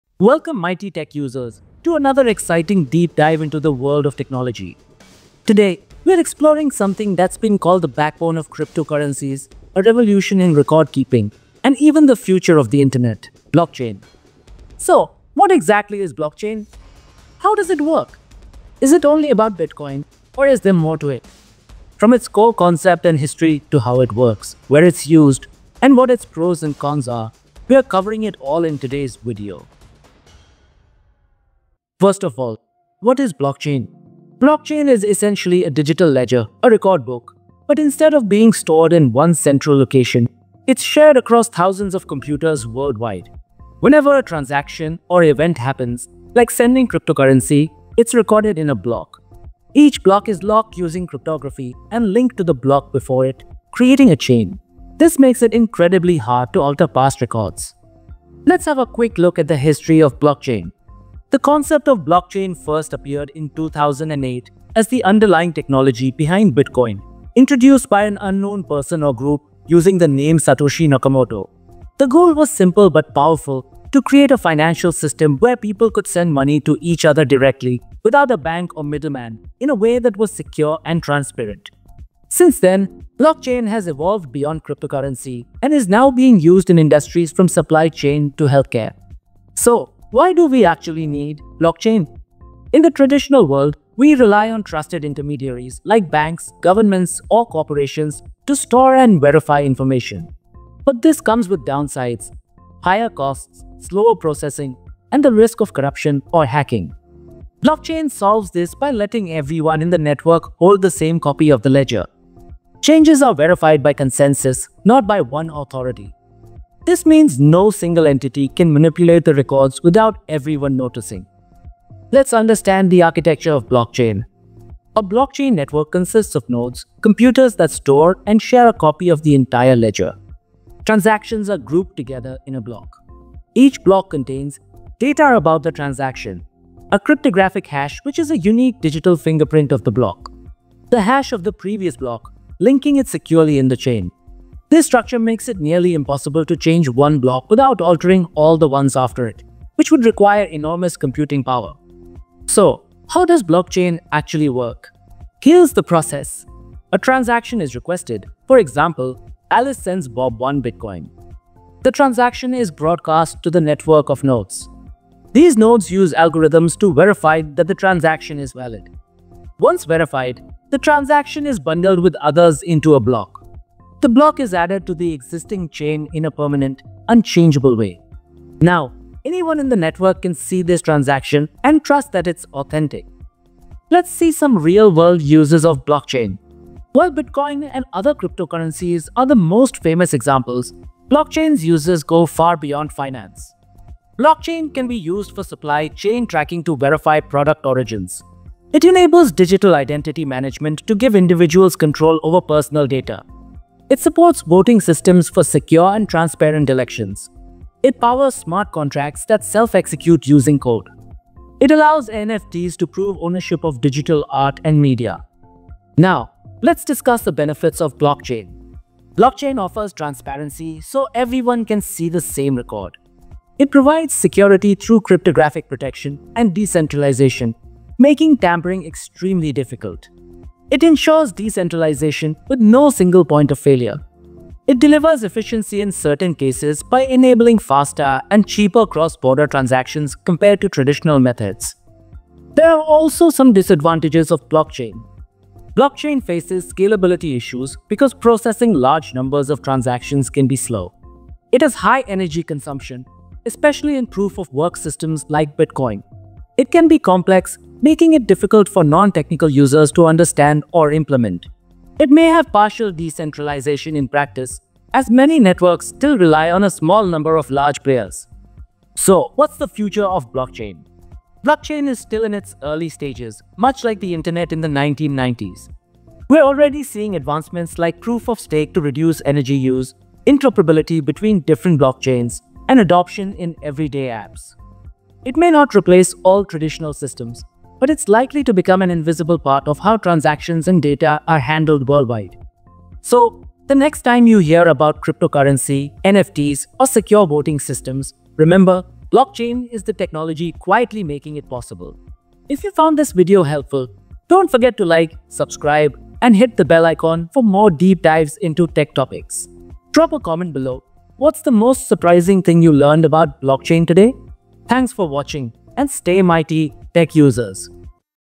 What is BLOCKCHAIN - History, Architecture, Uses & Benefits | Blockchain Explained
Hello Mighty Tech Users!
Discover everything you need to know about Blockchain technology in this detailed video guide. From understanding what blockchain is to exploring its history, architecture, working process, real-world applications, benefits, drawbacks, and even its future potential, this video covers it all.

📌 TIMESTAMPS
00:00 – Introduction
00:32 – What is Blockchain?
00:59 – History of Blockchain
01:30 – Why do we need Blockchain?
02:02 – Blockchain Architecture
02:32 – How Blockchain works?
03:04 – Real-world Blockchain uses
03:36 – Benefits of Blockchain
04:02 – Disadvantages of Blockchain
04:29 – Future of Blockchain
04:52 – Outro